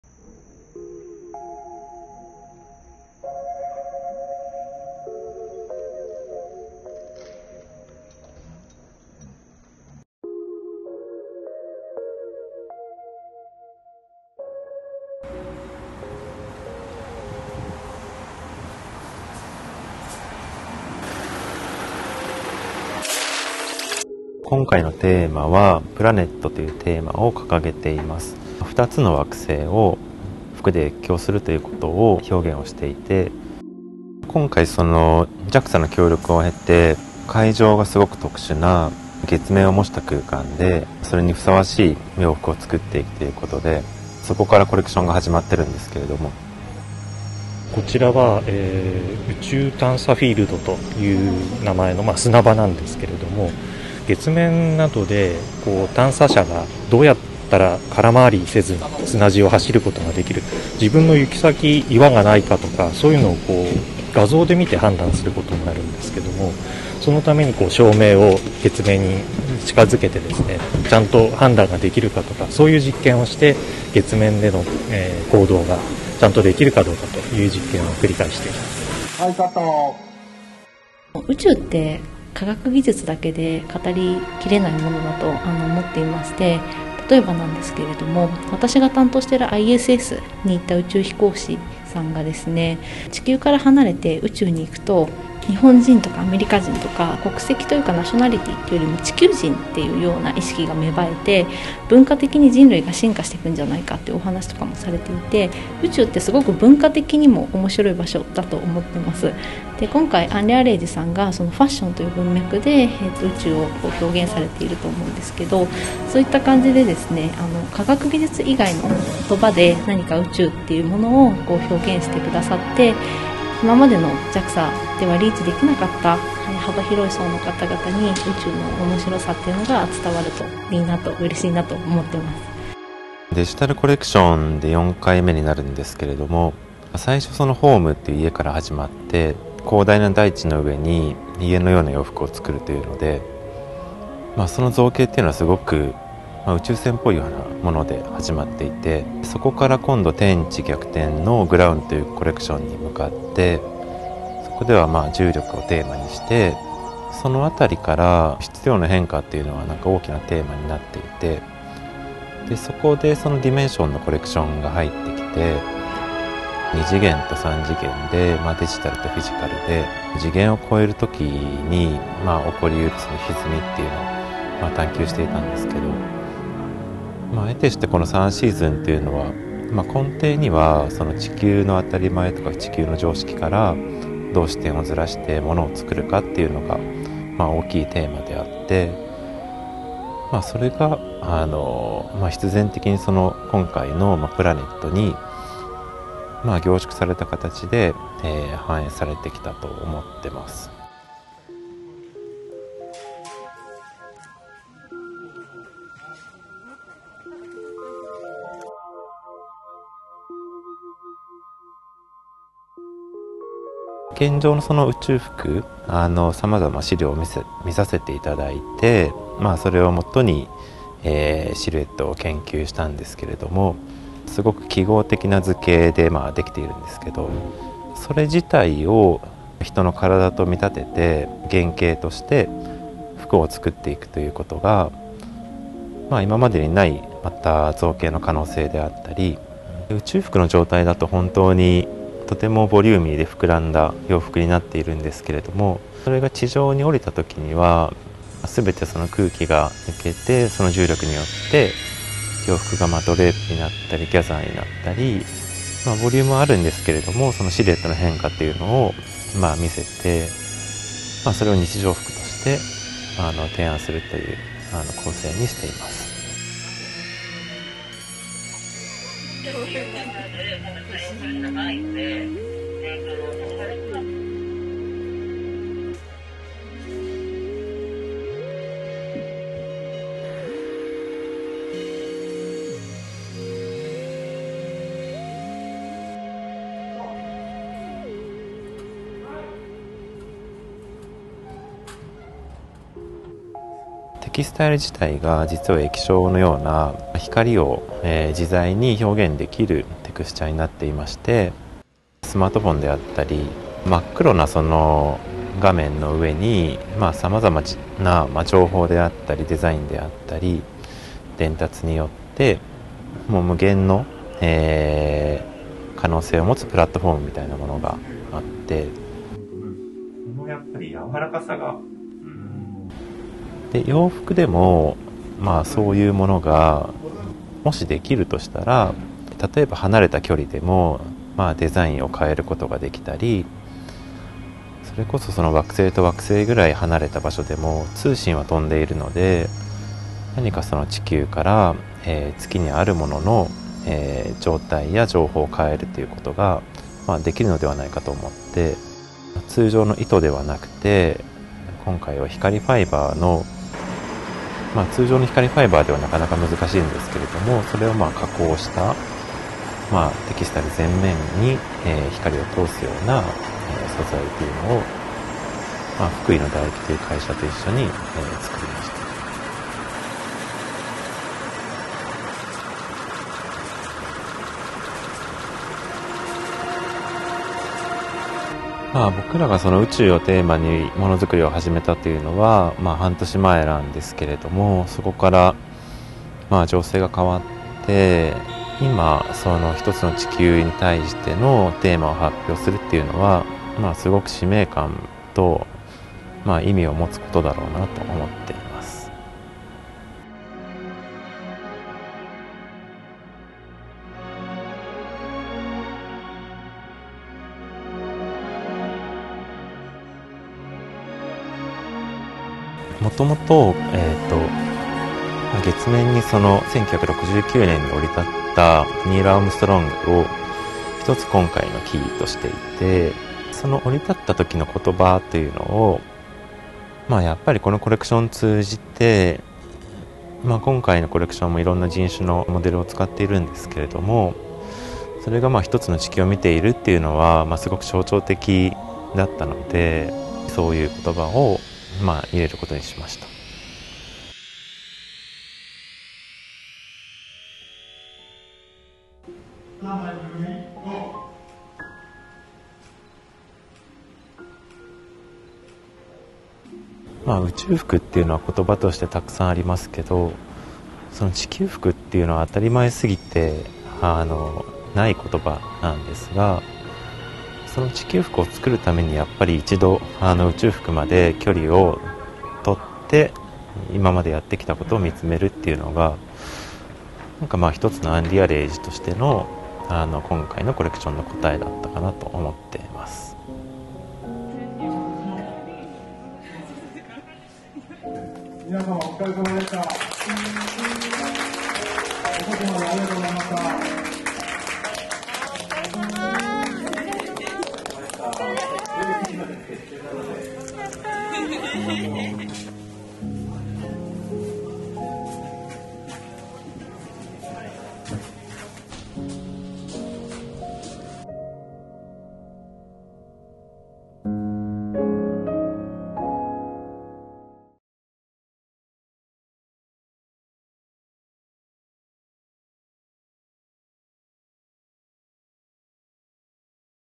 Anrealage Fall/Winter 2022 Behind the Scenes | Paris Fashion Week | VRAI Magazine
Go behind the scenes of the Anrealage FW22 fashion film created for PFW in February 2022.

Video courtesy of Anrealage